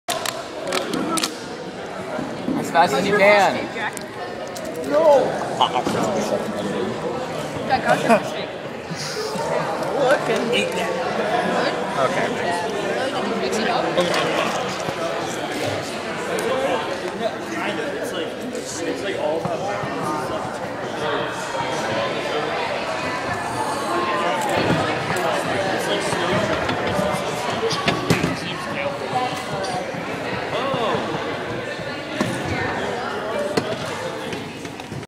2011 Oakwood XC lunch
starring hank beyer ben gallatins food and tommy lanes shirt